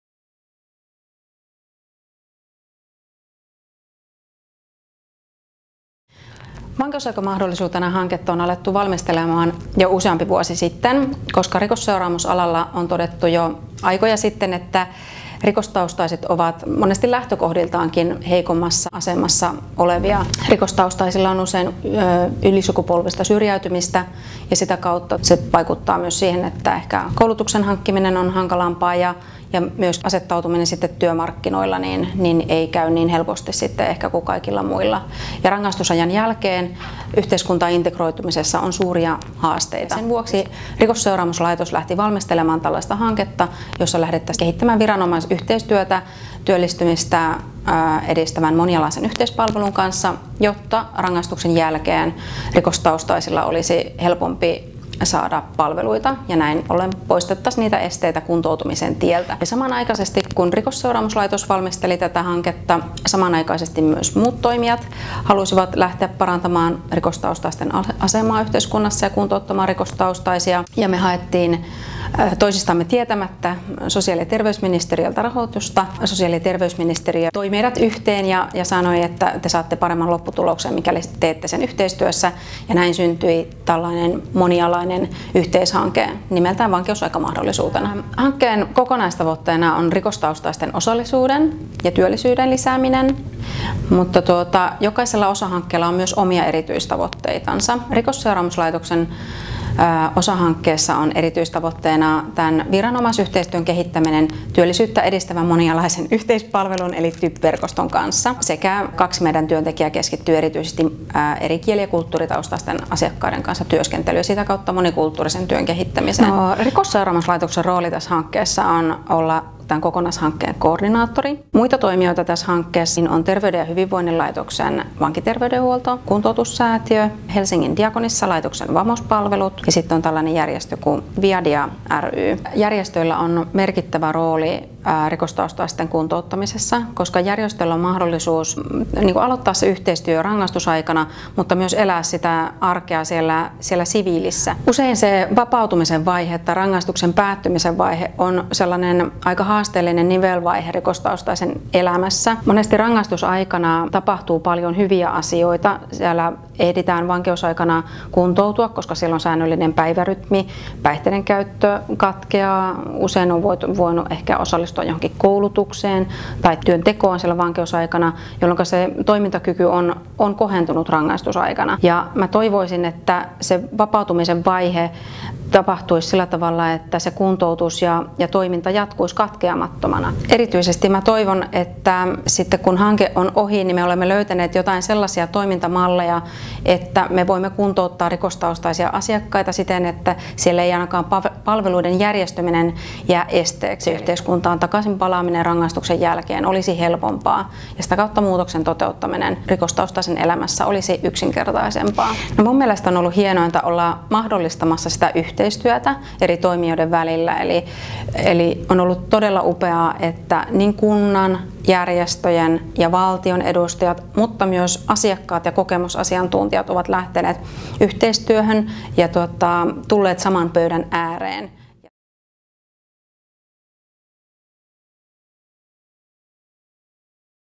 Vankeusaika mahdollisuutena – mistä on kysymys? (video 1/7)
”Jotta yhteiskuntaan takaisin palaaminen rangaistuksen jälkeen olisi helpompaa ja sitä kautta muutoksen toteuttaminen rikostaustaisen elämässä olisi yksinkertaisempaa.”
Projektipäällikkö Kati Kaarlejärvi kertoo videolla, miten monialaisella yhteistyöllä edistetään yhteiskunnallista turvallisuutta. Katso ja jaa, jos asia on mielestäsi tärkeä.

Kuvaus ja editointi: korkeakouluharjoittelija Elli Tervo